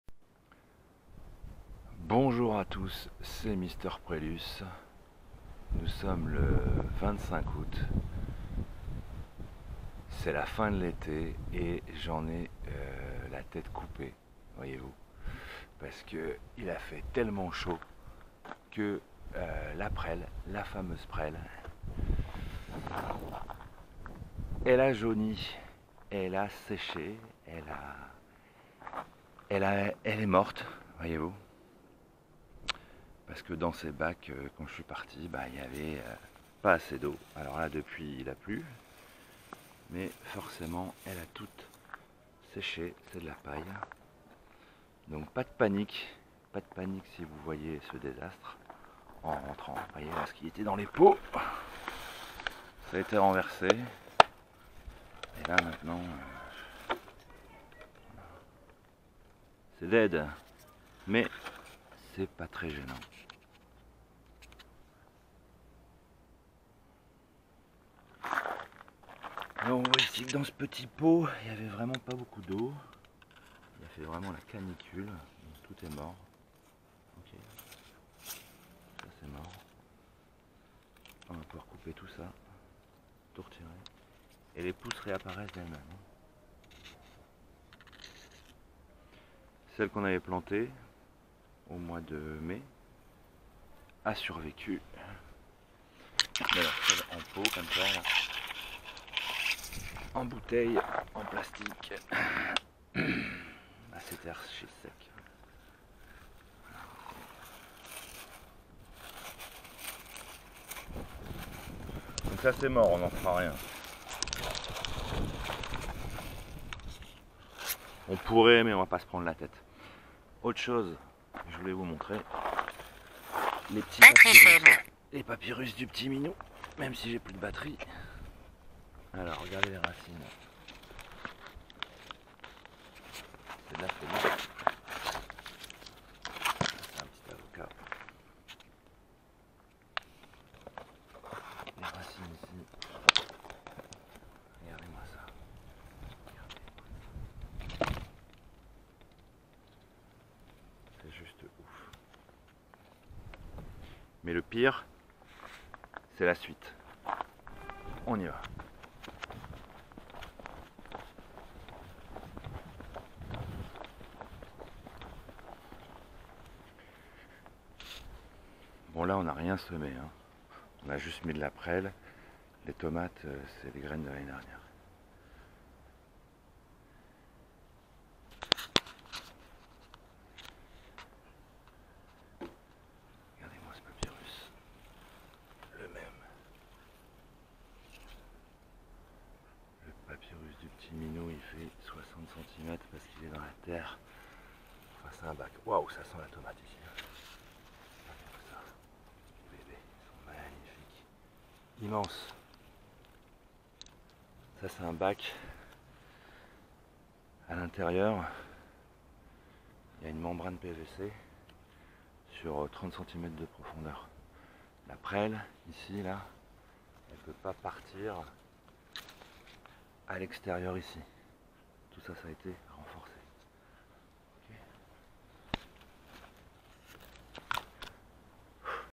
Episode N° 23 : Canicule. Quid du Papyrus ? La prêle aura t'elle survécu ?
Pendant l'épisode caniculaire de cet été, les prêles laissées en pots et en bacs n'ont pas toutes survécu…
4 semaines d'absence.
Dans cet #23 : la suite du papyrus "du petit minou" (à 3minutes 10 secondes) ...laisser en pot ou planter en terre (ici un bac) …